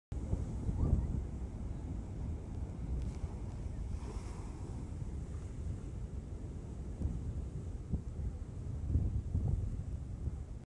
Huge waves from Typhoon Talim at Sunabe Seawall (2)
Watch as that couple walking on Seawall get drenched from wave splashing onto Seawall.